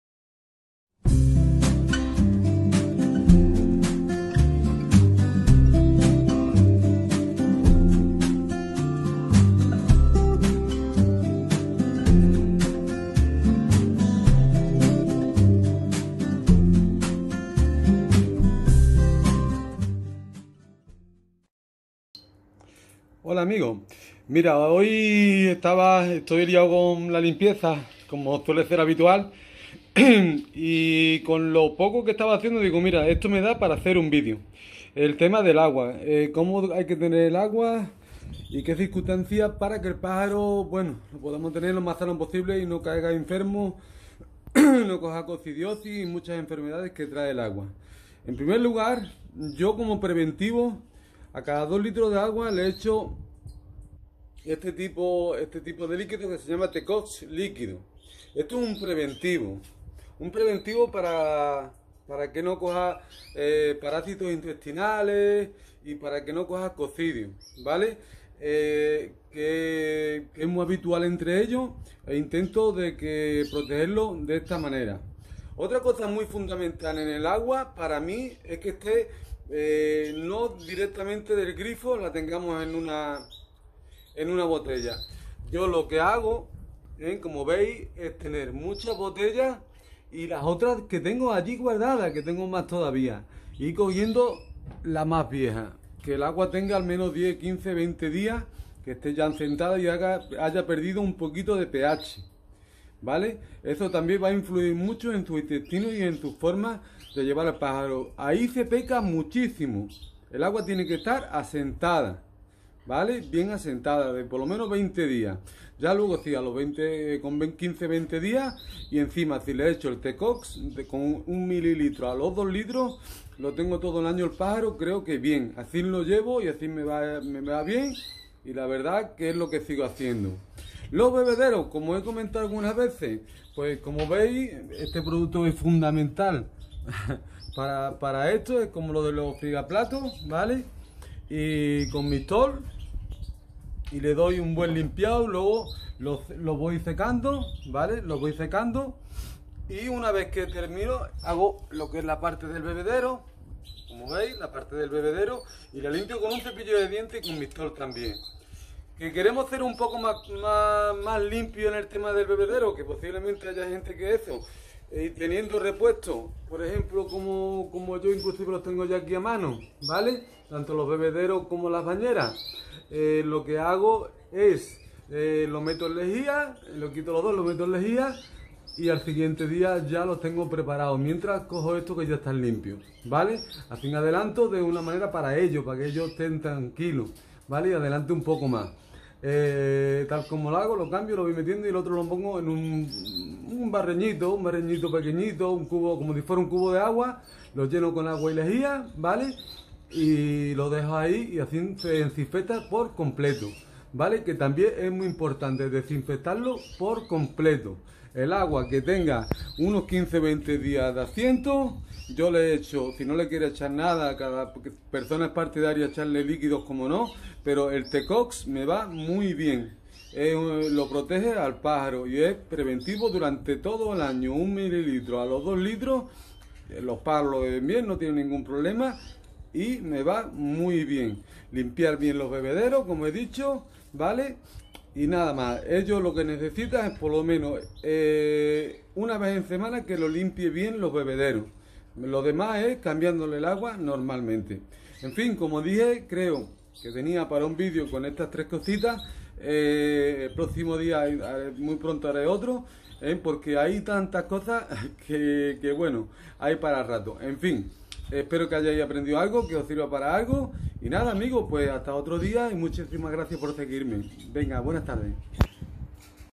Como darle de beber a los canarios
Hola amigos,otra vez con un video donde expongo todos los pasos que doy con el tema de el agua,tanto en higiene como en su mantenimiento, espero que os guste i sea ilustrativo y podamos aprender un poco mas cada día de estos animales tan maravilloso 👏